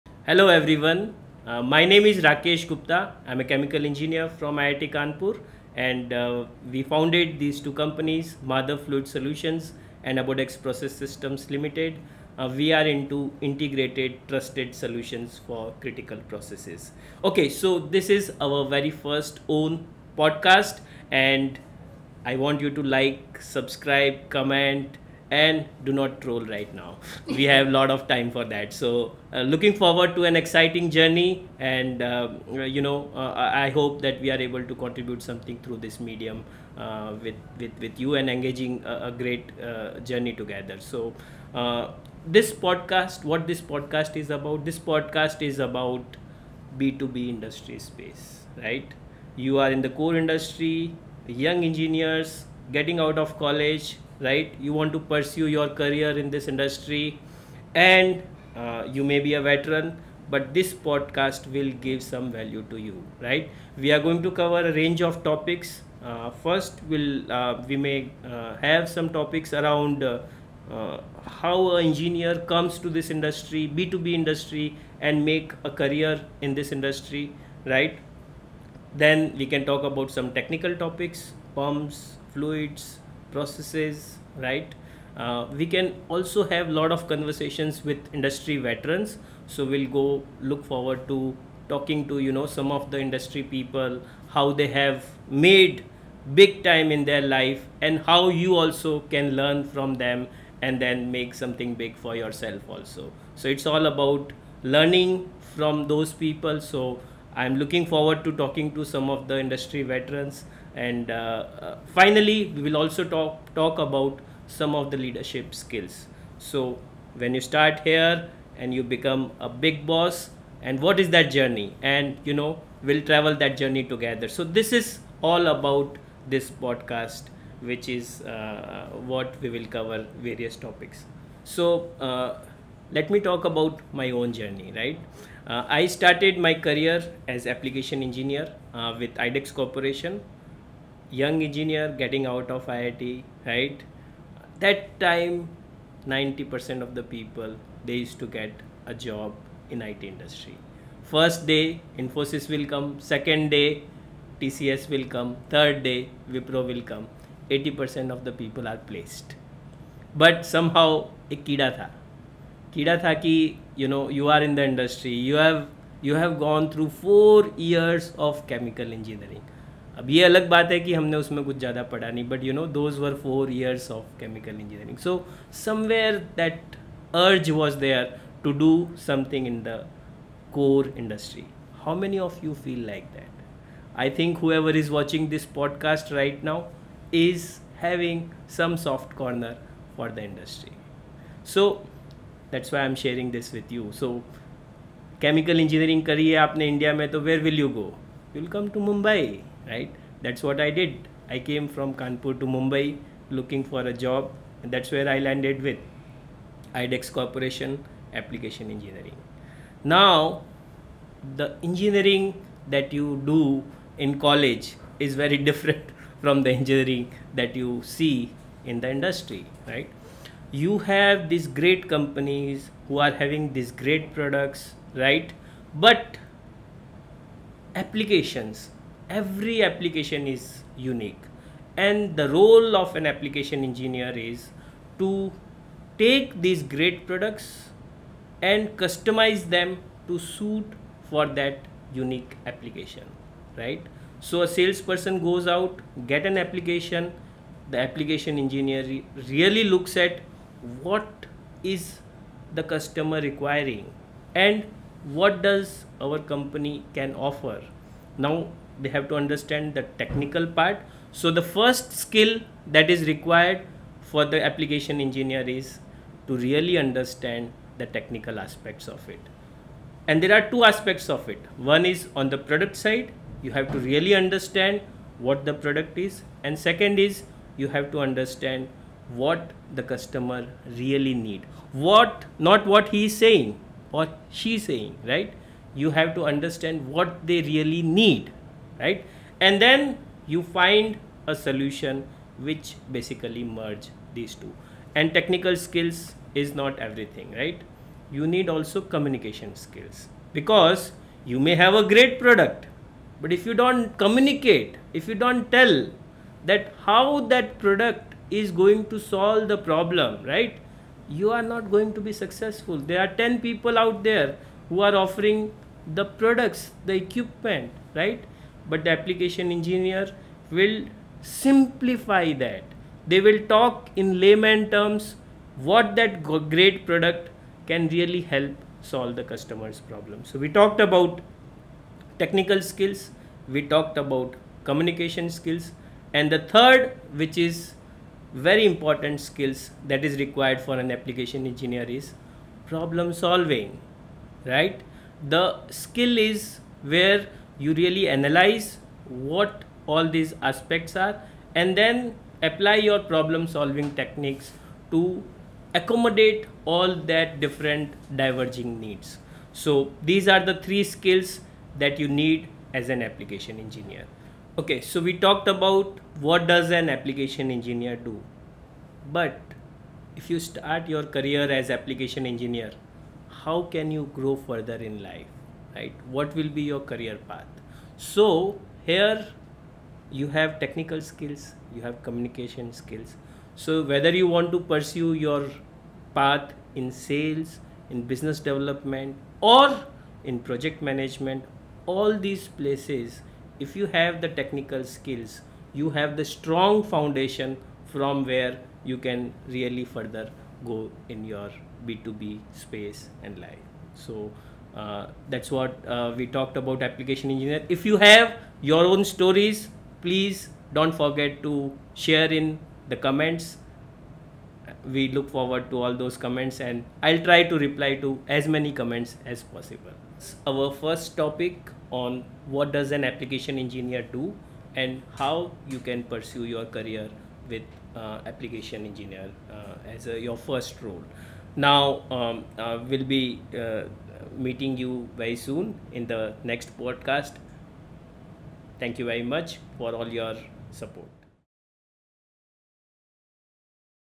Why Application Engineering is a great career choice?
In this maiden video, we introduce the topics that our podcast will explore.

We delve deeper into the  role of an application engineer do and what it takes to succeed in this field.  Beyond technical skills, successful application engineers need strong communication and problem solving abilities.